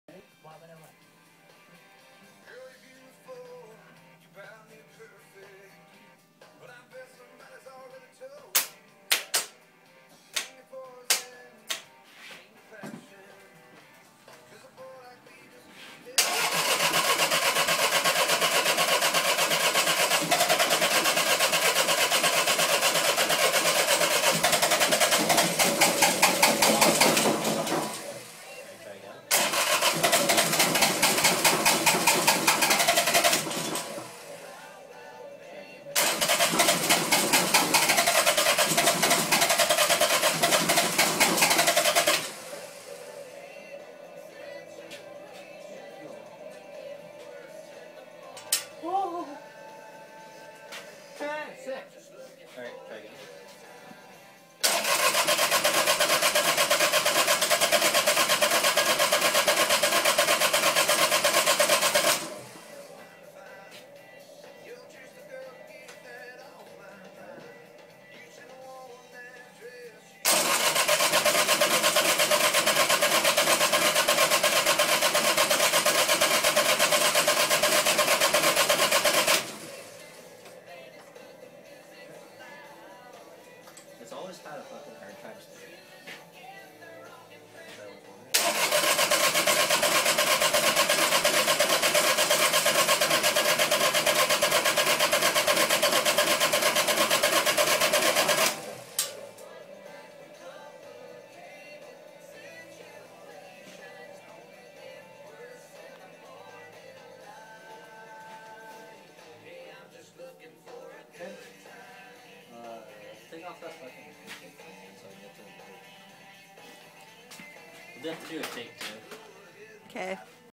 3 Cylinder Yanmar Diesel
Removed from a ThermoKing refrigeration unit to power a woodsplitter